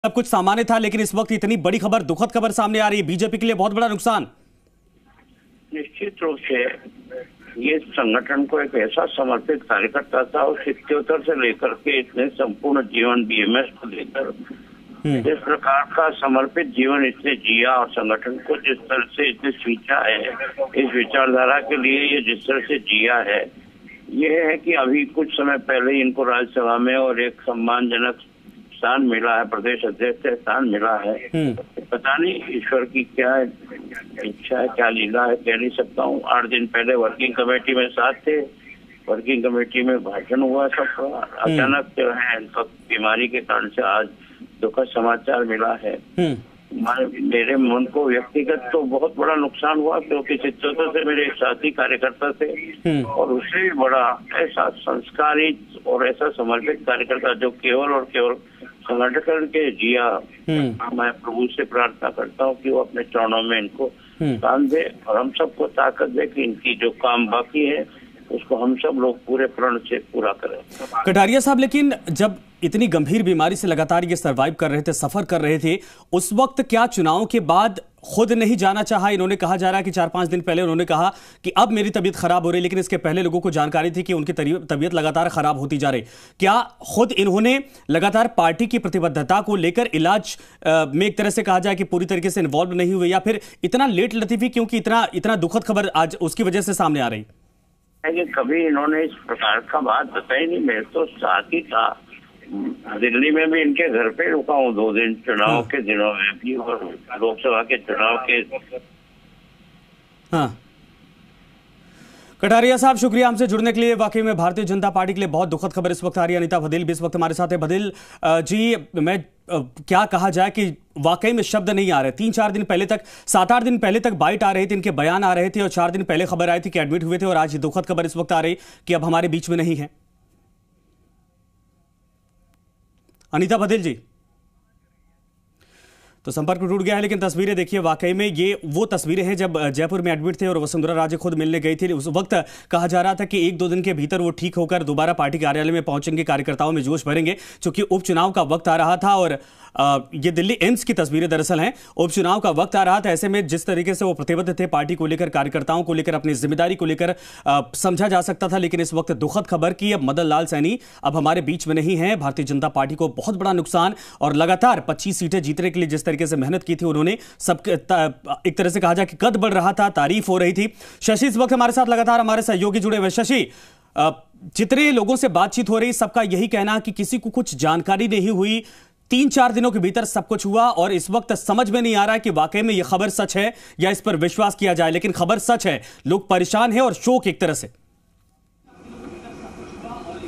MadanLalSaini के निधन पर, नेता प्रतिपक्ष gulabchandkatra ने कहा..
MadanLalSaini के निधन पर, नेता प्रतिपक्ष gulabchandkatra ने ZeeMedia से फोन पर कहा - मुझे यकीन नहीं हो रहा इस समाचार पर... पता नहीं ईश्वर की क्या इच्छा है ... अभी आठ दिन पहले ही वर्किंग कमेटी में उनका भाषण हुआ था, यकीन नहीं होता अब वह हमारे साथ नहीं हैं'